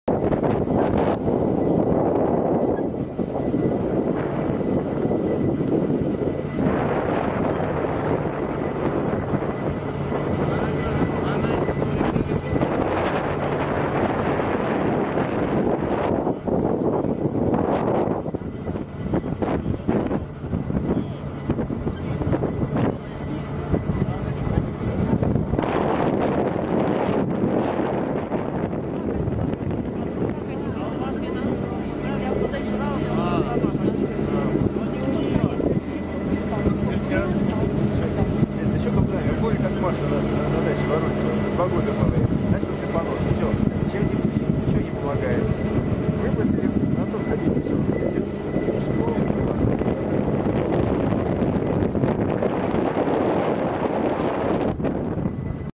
по Алтаю
На автомобиле газ 66 по алтаю